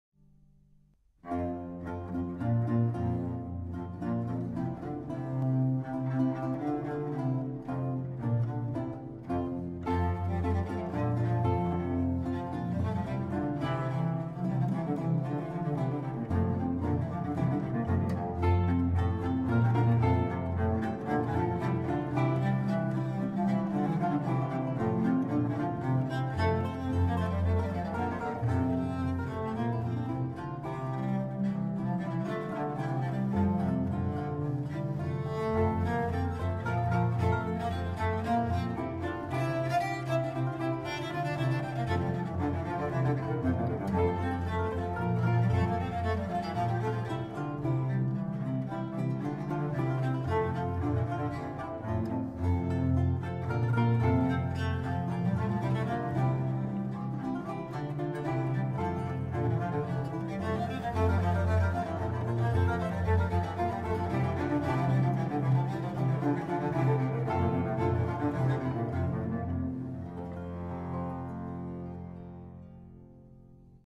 Amor con Fortuna Solo
Ricardo Graziano in Val Caniparoli's Amor con Fortuna.